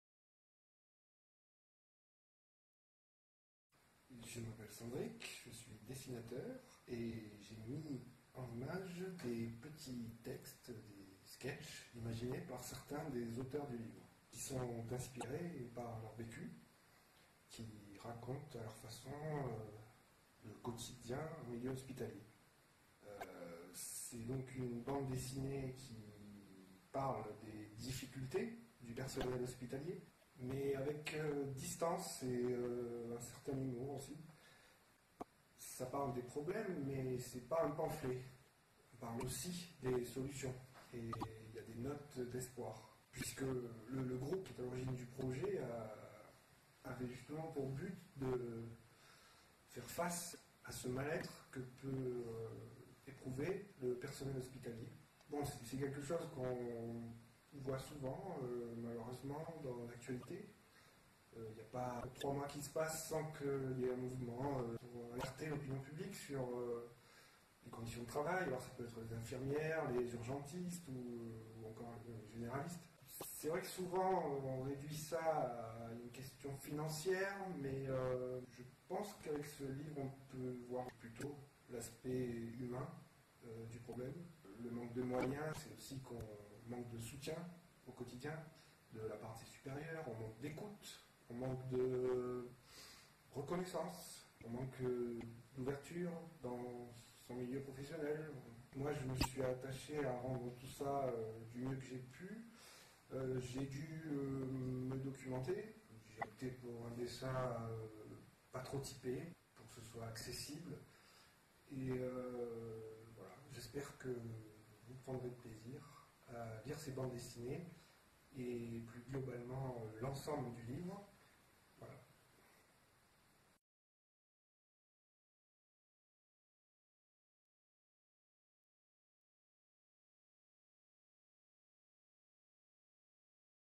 Michèle MAURY, Patrice TAOUREL - Les médecins ont aussi leurs maux à dire - par Sandrik DMITRIEFF
Les médecins hospitaliers n’ont pas l’habitude de se plaindre. Leur vocation est de soigner les autres et non de se préoccuper de leur propre santé physique et psychique. Et pourtant burn-out, suicides et accidents professionnels ne sont pas rares dans la communauté médicale.

Au CHU de Montpellier, à la suite de la mort tragique d’un jeune médecin, un groupe « bien-être au travail » s’est créé dans l’objectif de valoriser l’expression du vécu du métier plutôt que de rechercher les coupables. Les difficultés relationnelles au sein des équipes médicales et le sentiment de non-reconnaissance sont apparus comme facteurs principaux de souffrance. Ce changement de représentation sociale du médecin à l’hôpital a conduit à des actions concrètes : amélioration des relations médecins /directeurs, ouverture d’une consultation spécifique pour les médecins, rédaction d’une charte des relations en équipe…

À l’ère d’un management centré sur la performance et l’inflation des messages électroniques, cette expérience, qui se poursuit aujourd’hui, montre combien les échanges « en chair et en os » entre collègues sont les garants d’une coopération efficace et de la santé au travail. Médecins et directeurs en témoignent. Une bande dessinée illustre quelques aspects de leur vie dans l’hôpital du XXIe siècle.